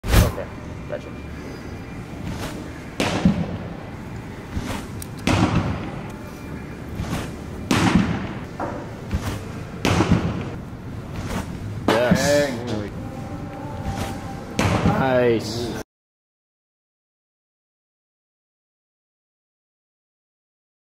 Jeurys Familia Improving His Splitter
Jeurys Familia working on refining his splitter this past off-season.